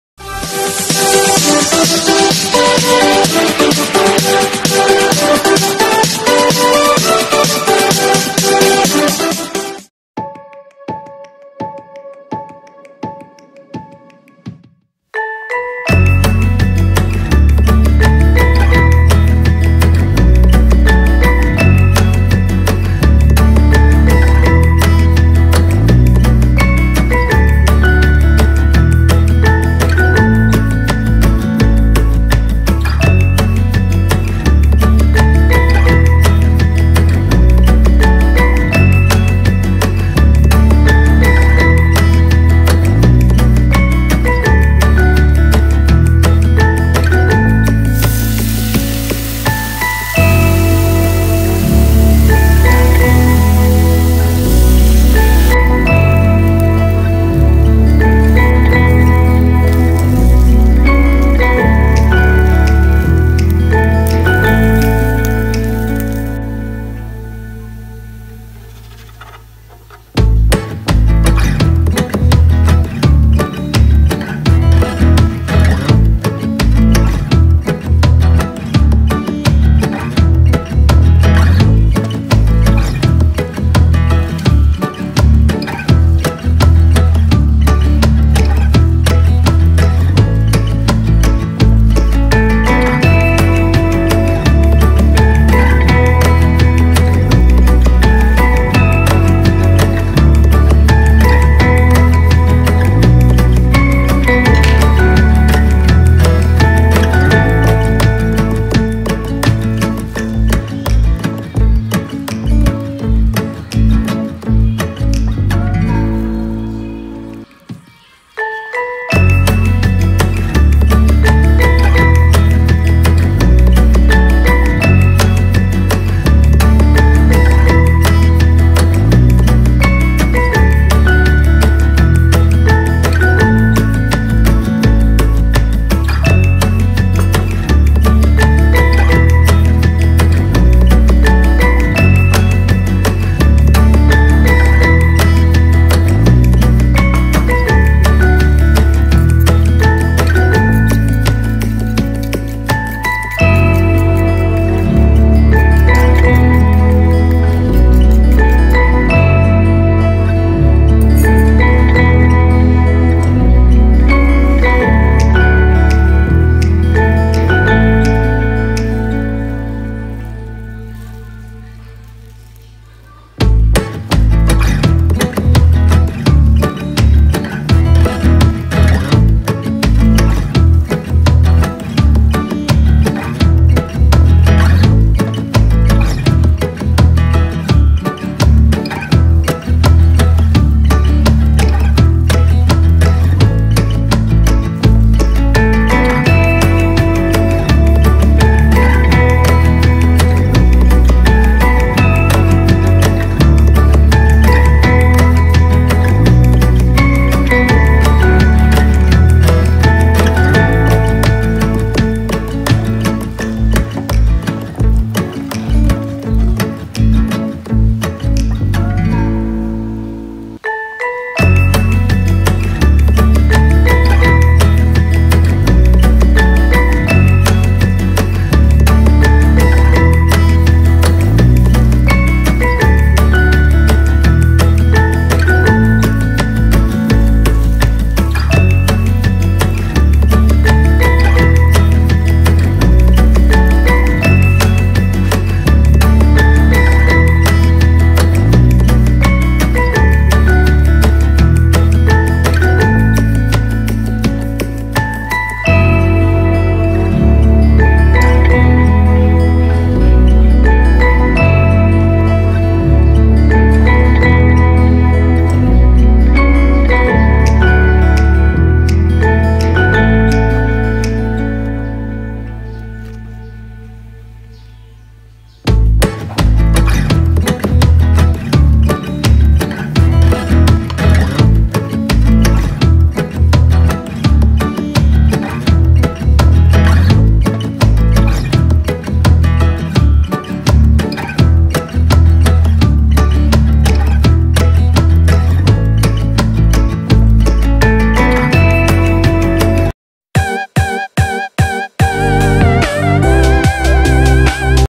EASY HOMEMADE CHESSY HAM TOAST | Filipino Style
Chessy Ham Toast(Ingredients)
4 pcs loaf bread
2 slices of cheese
2 slices of ham(pan fried)
3 eggs
1/2 tsp salt
1/2 tsp sugar
Butter